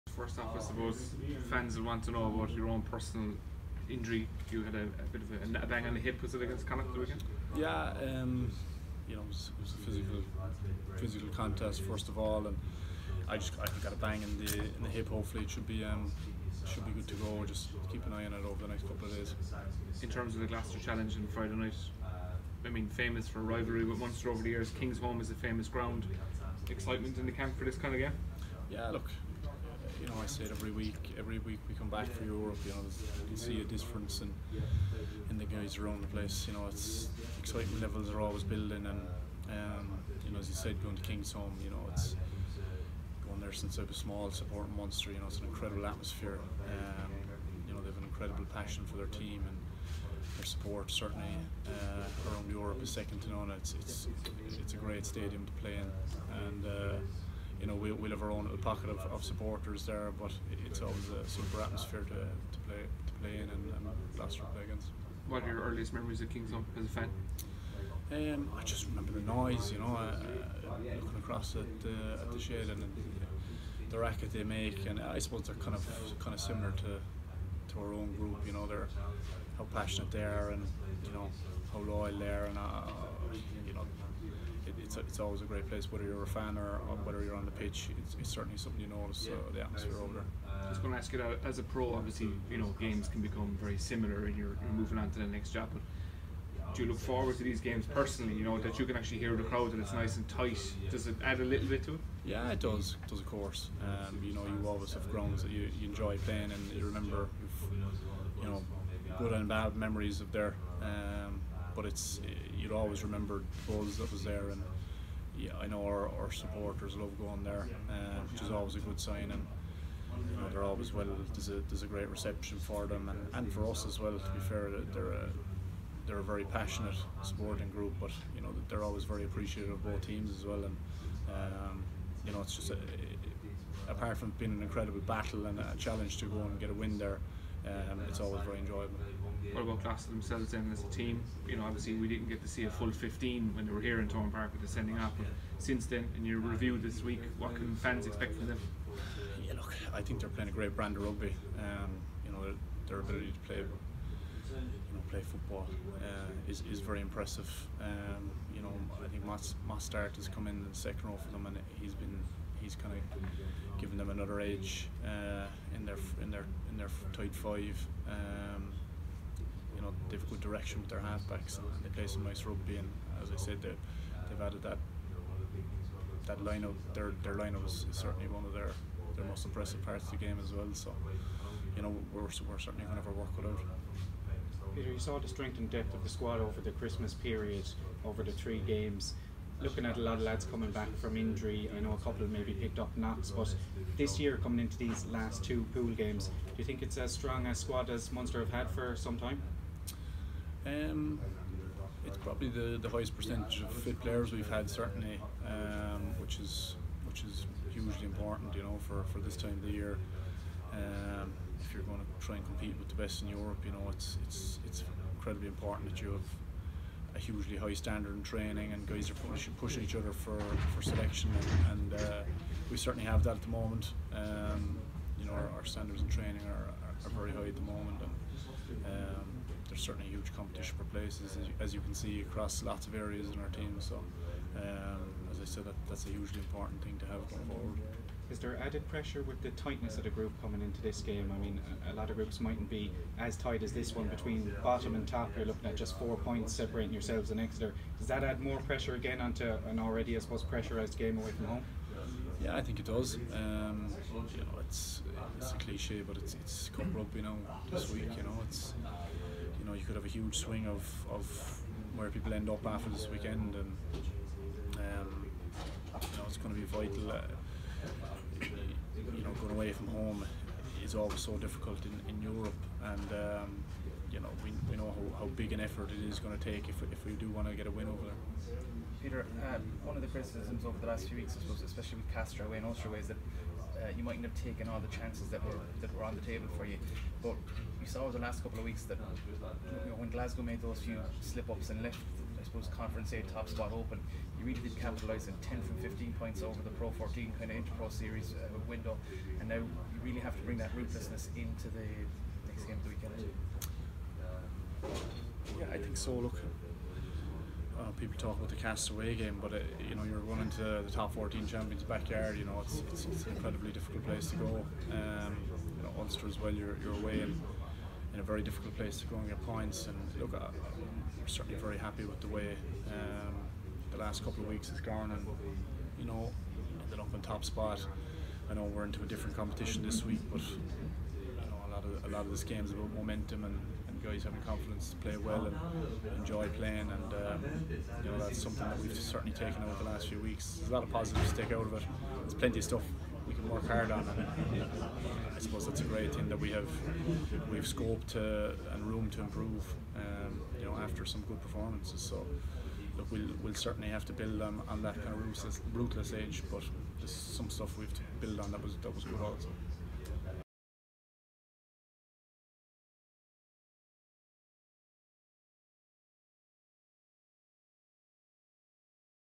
Peter O'Mahony on why he's especially relishing Munster's Kingsholm trip
Munster captain Peter O'Mahony looks ahead to the province's Heineken Champions Cup clash with Gloucester Rugby and reflects on the win over Connacht